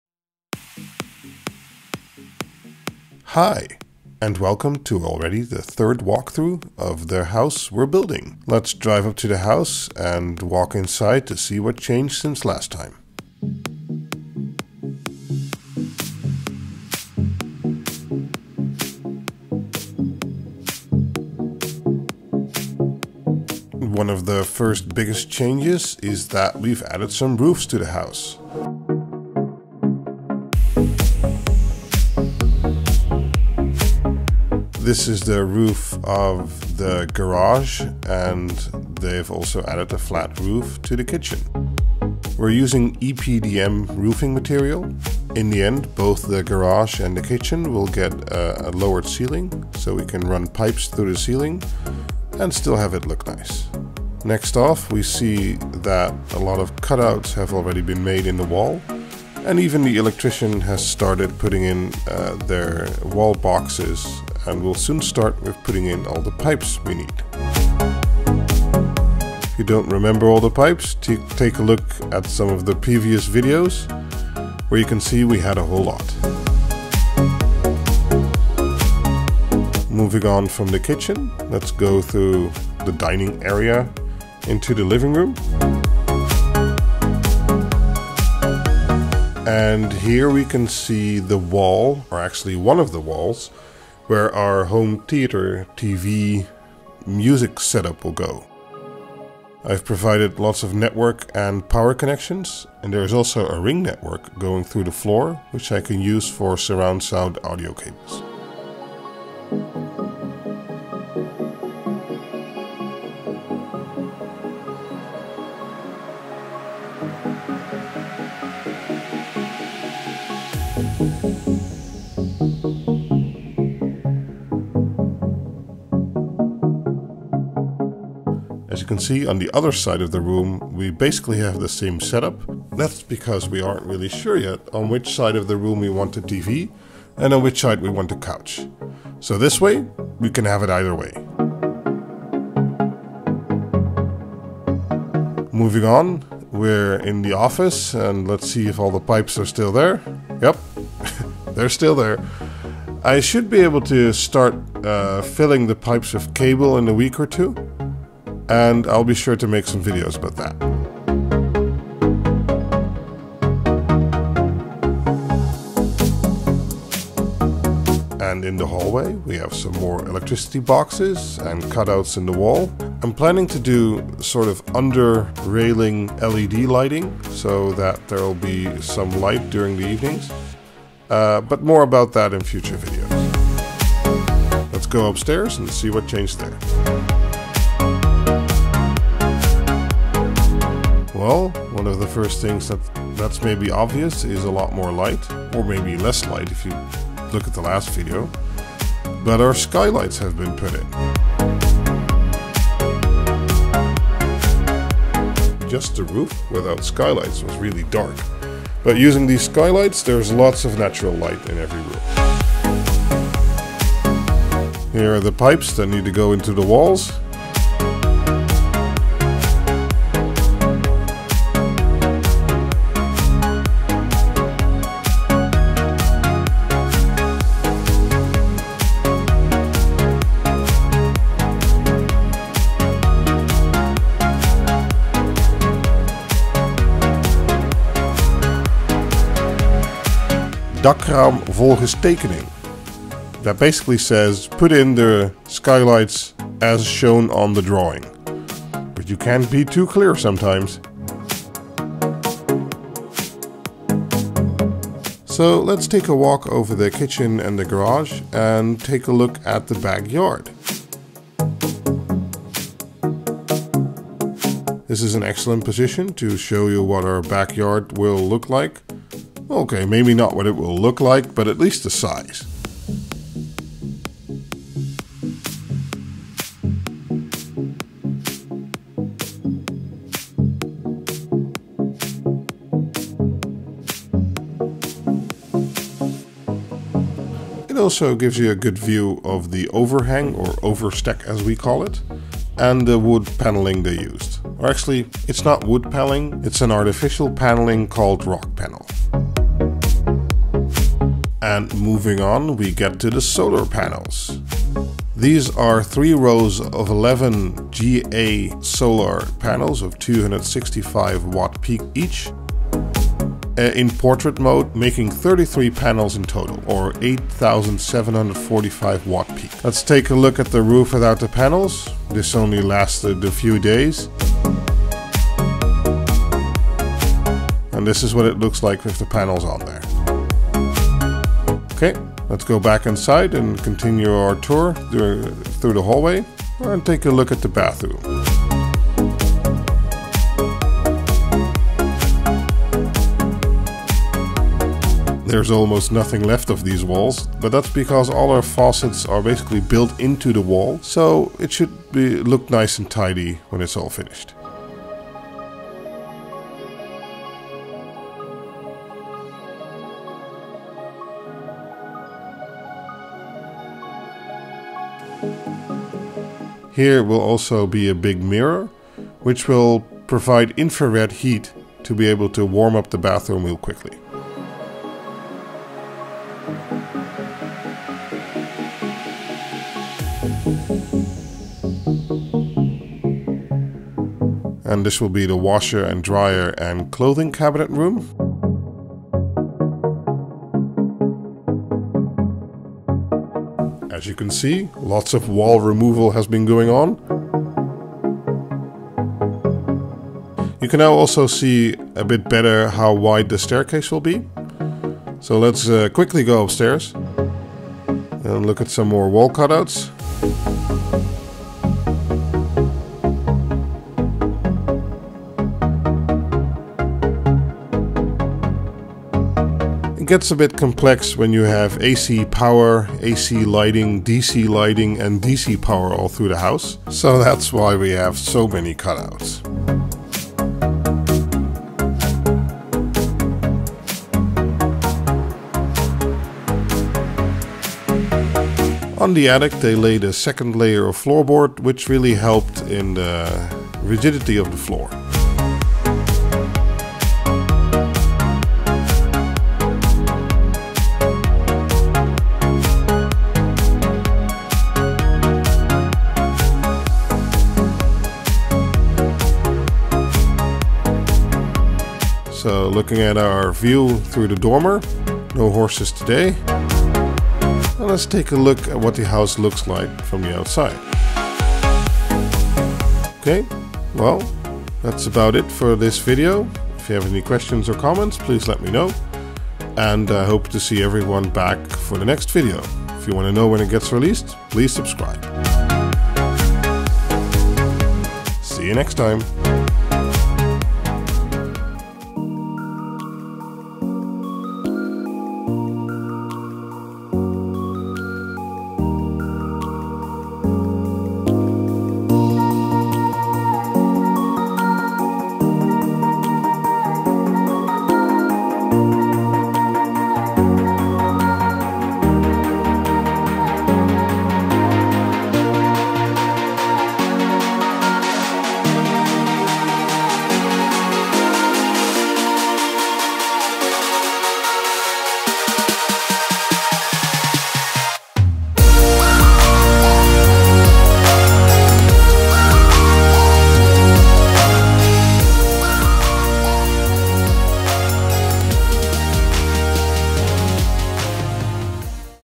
Home Networking: Third walkthrough
░▒▓[Description]▓▒░
A lot has happened since the last walkthrough so time for another video. This video has some technical content in it and future videos will get a lot more once the house is more finished!

░▒▓[Music]▓▒░
Intro&Outro